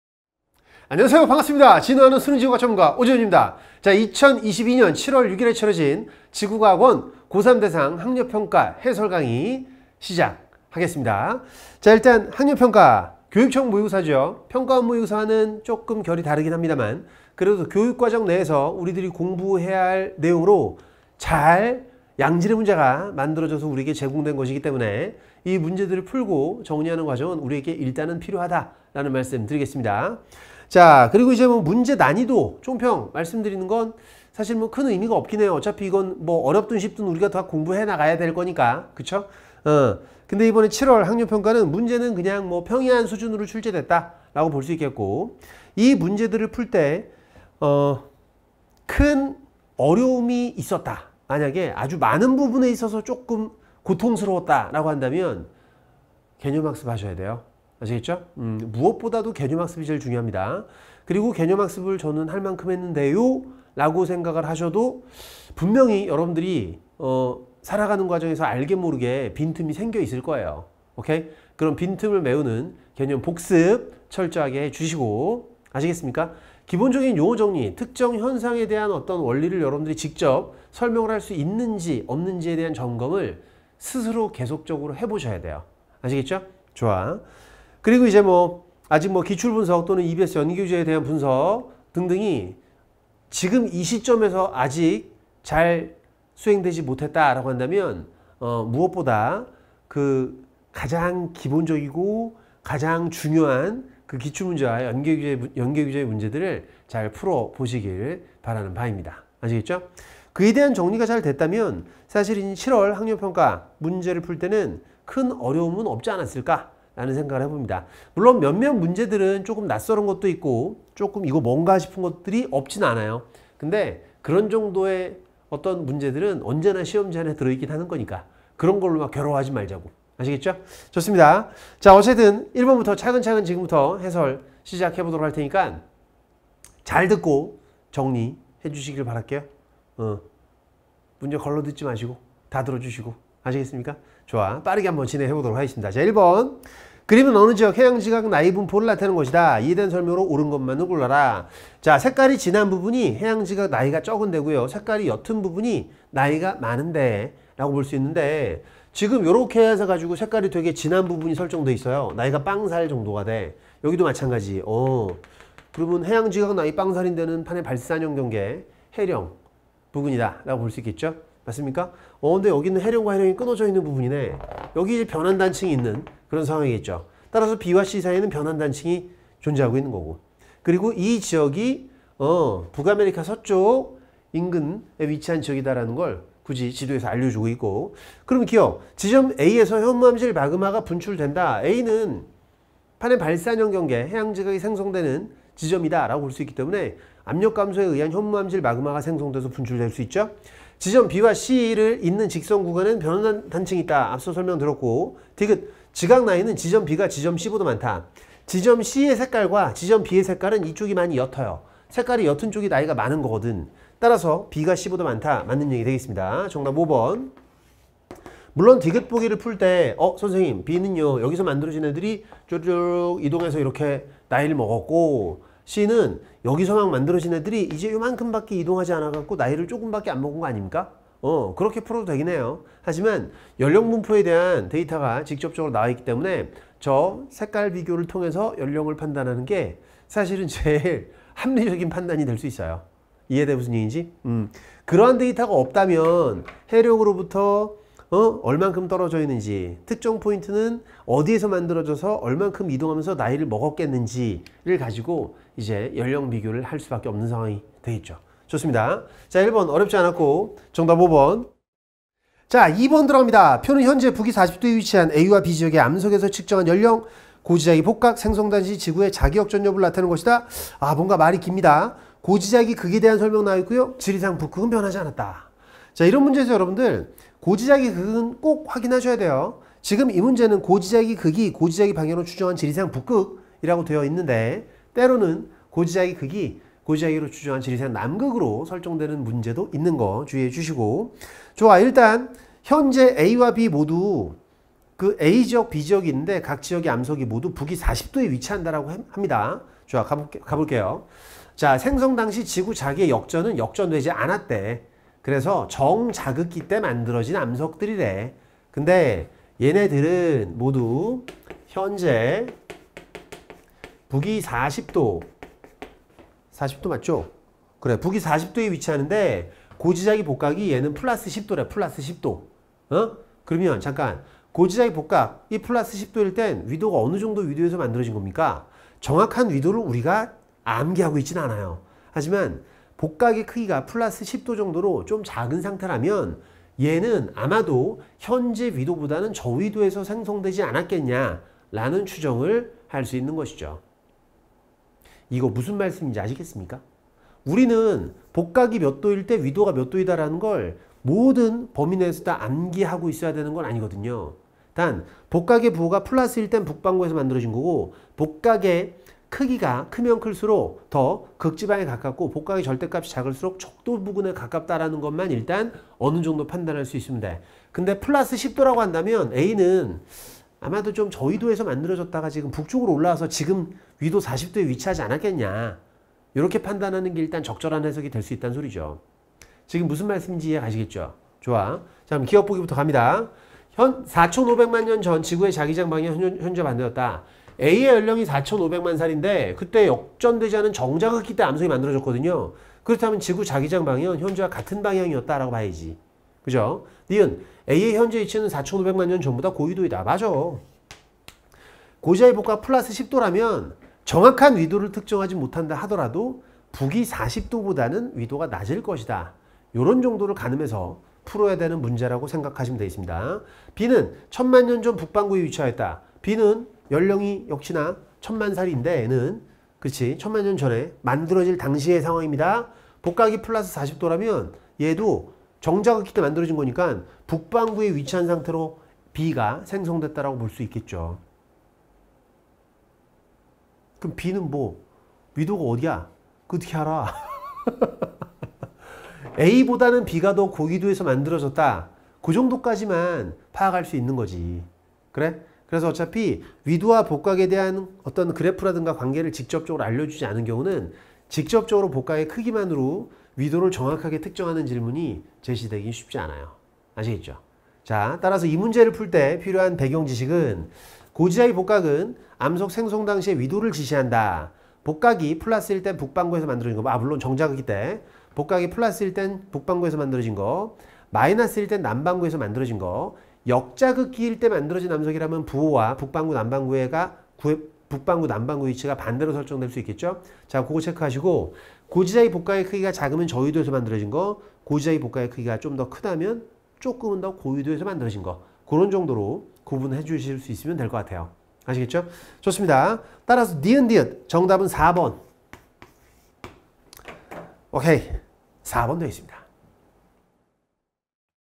[메가스터디] 지구과학 오지훈쌤 - 고3 7월 학평 지구과학I 해설강의
오지훈 선생님의 2023학년도 7월 학평 고3 지구과학I 해설강의가 업로드 되었습니다.

- 지구과학 오지훈 선생님을 더 알고 싶다면?